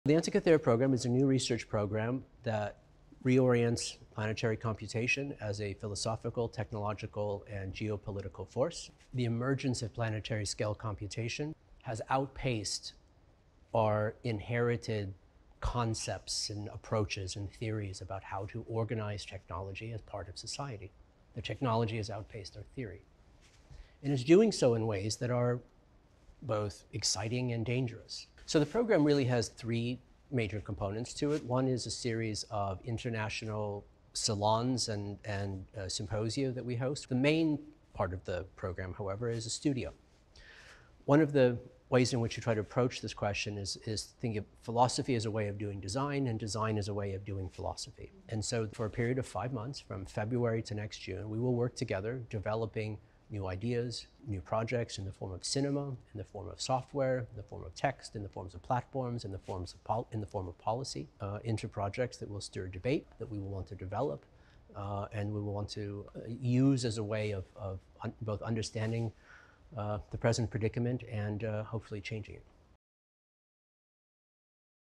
What Is Antikythera?
Benjamin Bratton, Director of the Antikythera program, discuss this new project.